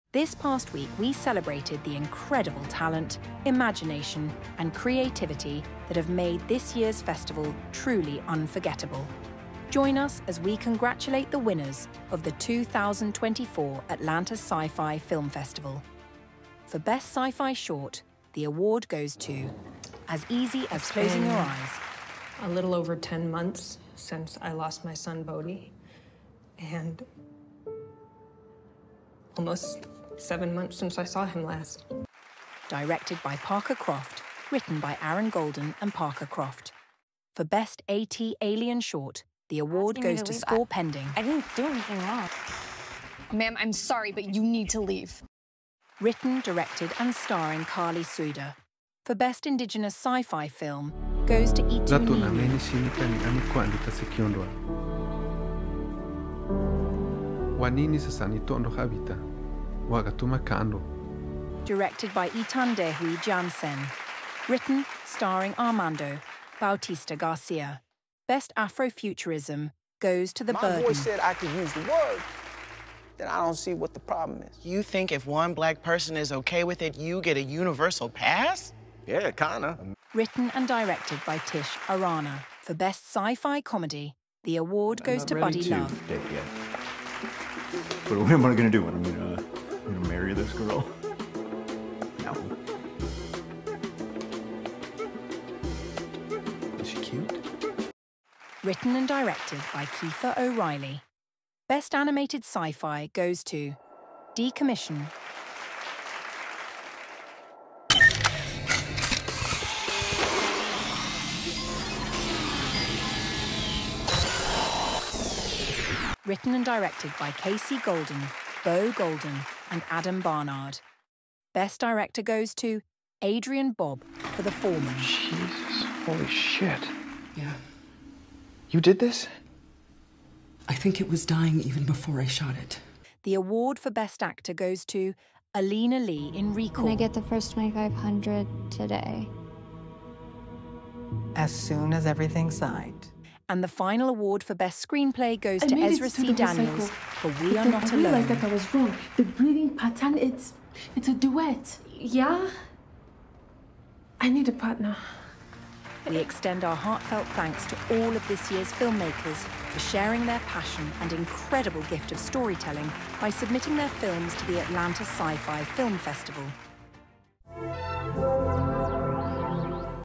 2024 Award Winners!
Join us in celebrating the incredible talent, imagination, and creativity that have made this year’s festival truly unforgettable. Here are the winners of the 2024 Atlanta Sci-Fi Film Festival!
We extend our heartfelt thanks to all of this year’s filmmakers for sharing their passion and incredible gift of storytelling by submitting their films to the Atlanta Sci-Fi Film Festival.